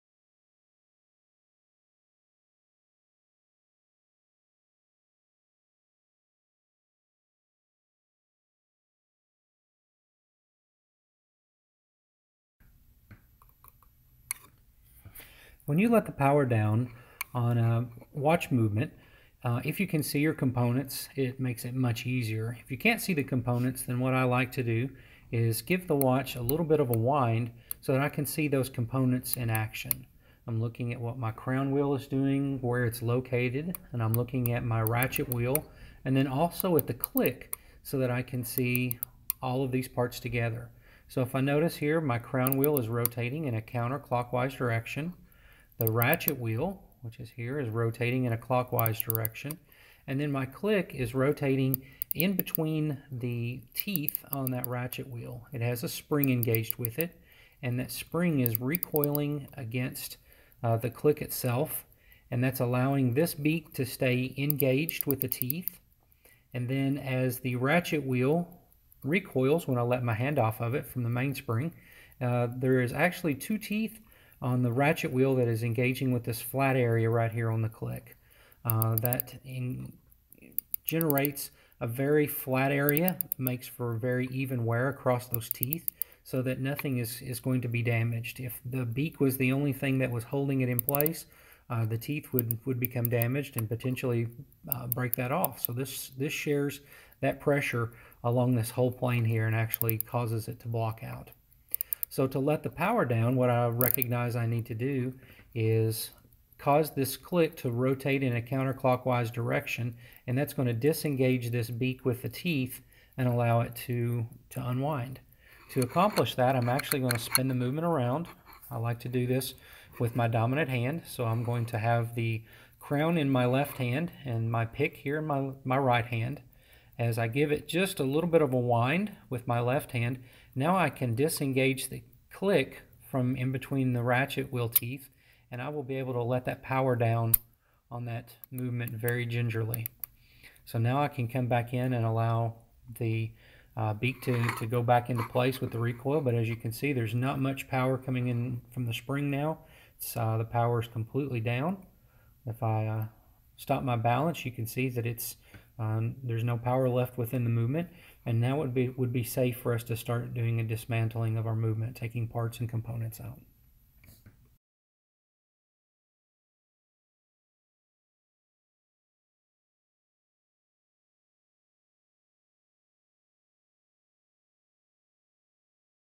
Let Down the Power of a Mechanical Watch
Before you can safely disassemble a mechanical watch you need to make sure there is no power on the mainspring. In this video the certified watchmakers at the American Watchamkers-Clockmakers Institute demonstrate a safe and professional way to release the power using the ETA 6497 movement.